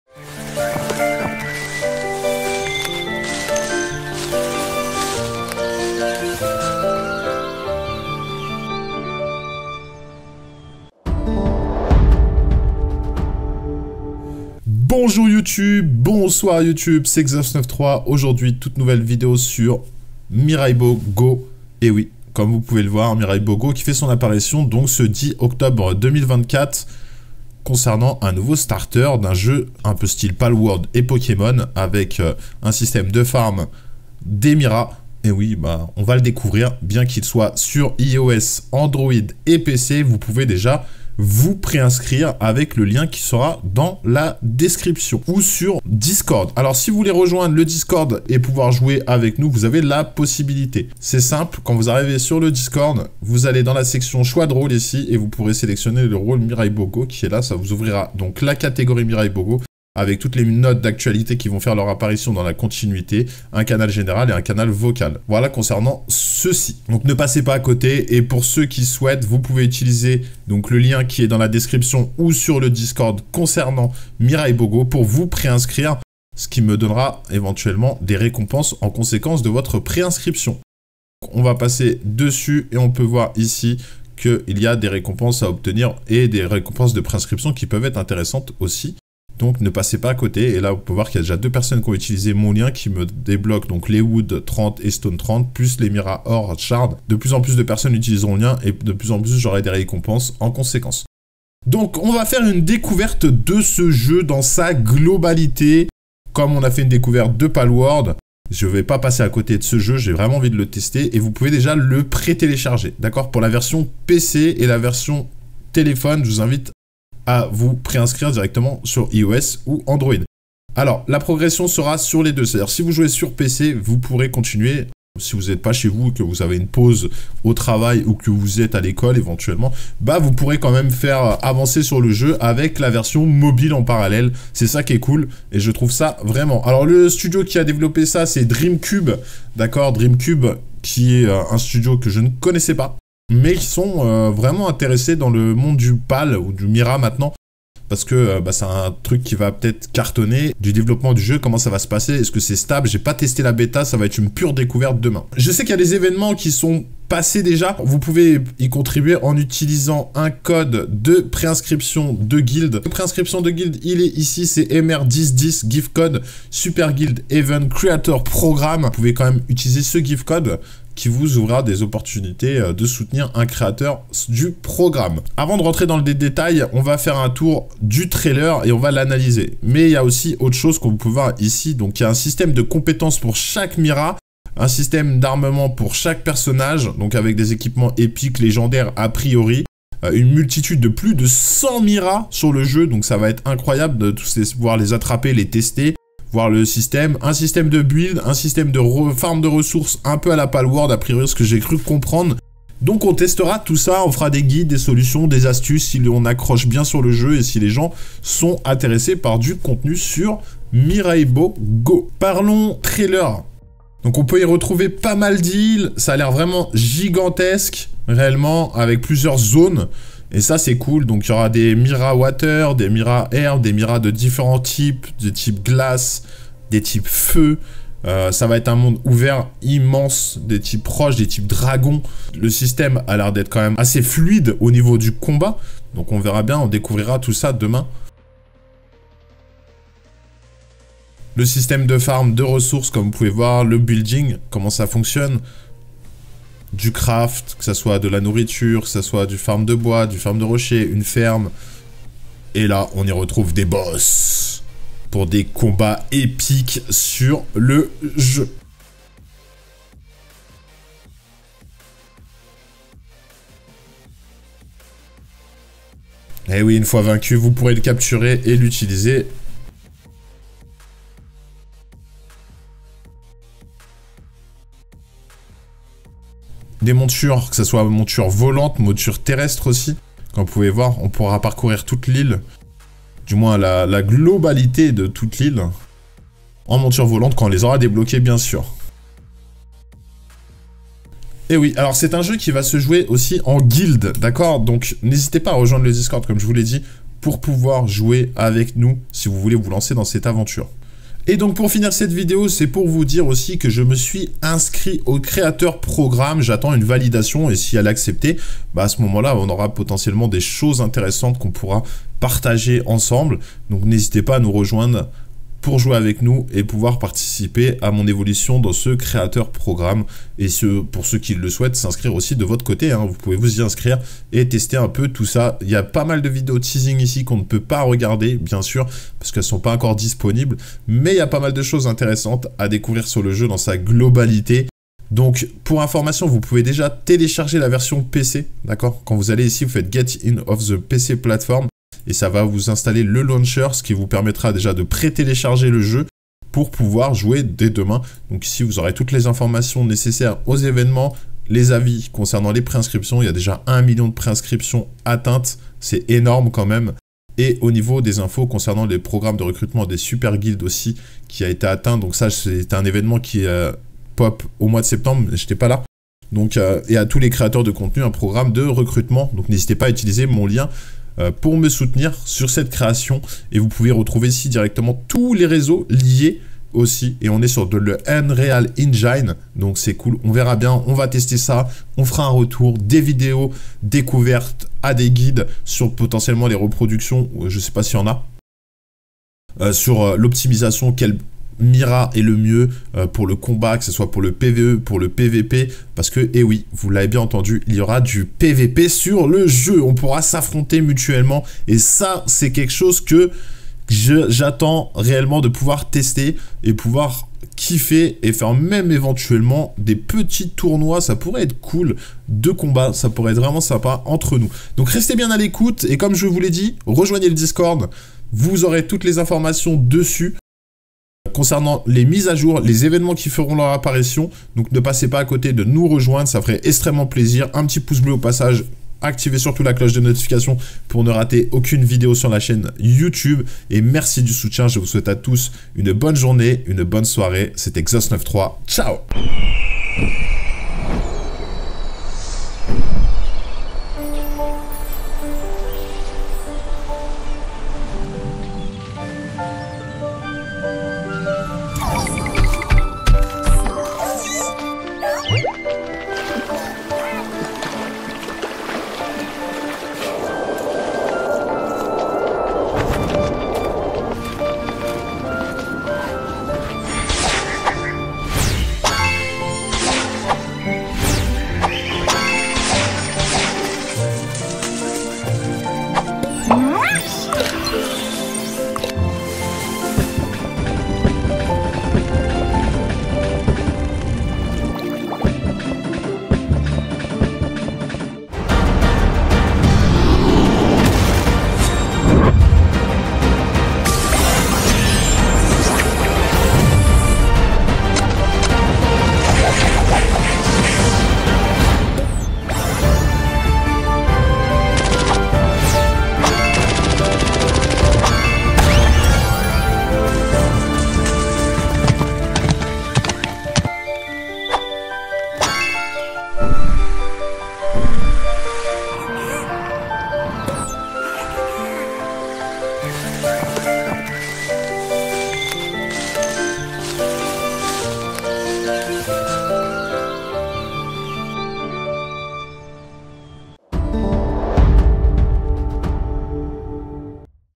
MIRAIBO GO : Jeu mobile Palworld sur Android, iOS et PC ! Tout ce que vous devez savoir !
Découvrez Miraibo GO, le nouveau jeu de survie en monde ouvert inspiré de Palworld et Pokémon, dont la date de sortie vient d'être annoncée pour le 10 octobre sur iOS, Android, et PC ! Avec déjà plus de 1 million de préinscriptions, les joueurs pourront capturer et utiliser des créatures appelées Miras pour explorer, construire des bases, et combattre. Le jeu propose aussi des modes multijoueur en coopération et en PvP, avec des mondes dédiés aux guildes et au jeu solo.

Dans cette vidéo, je vous donne tous les détails sur la sortie de Miraibo GO, les récompenses des préinscriptions et ce qui vous attend à la sortie. Préparez vous à un univers captivant avec plus de 100 créatures uniques, des fonctionnalités en ligne et bien plus encore !

⚔️ Rejoignez aussi notre communauté sur Discord pour discuter, partager des astuces, et organiser des sessions multijoueur ensemble !